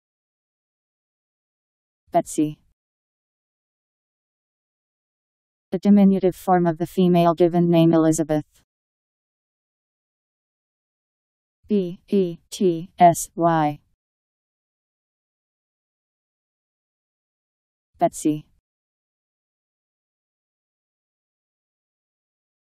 Betsy Meaning
Video shows what Betsy means. A diminutive form of the female given name Elizabeth..  Betsy Meaning. How to pronounce, definition audio dictionary. How to say Betsy. Powered by MaryTTS, Wiktionary